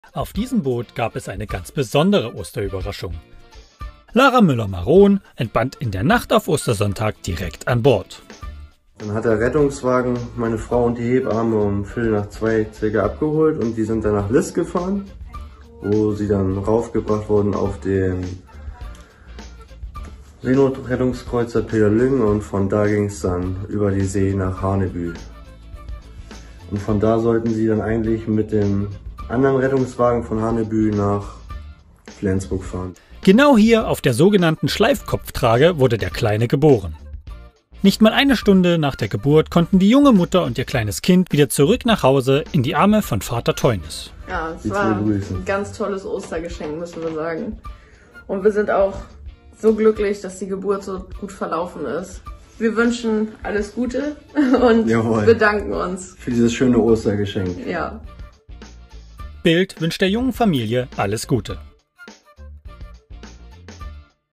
Baby Bosse wird auf Seenotkreuzer geboren: So lief die Geburt ab | Nordsee
Als die Fruchtblase platzte, begann eine turbulente Nacht für Lara Müller-Maron (25) und die Besatzung des Seenotkreuzers „Pidder Lüng“. Sohn Bosse kam überraschend auf der Nordsee zur Welt – zwischen Sylt und Dänemark.

Die Geburt auf hoher See überstanden Mama Lara Müller-Maron und Baby Bosse sehr gut, waren schnell wieder vereint mit Papa Teunis.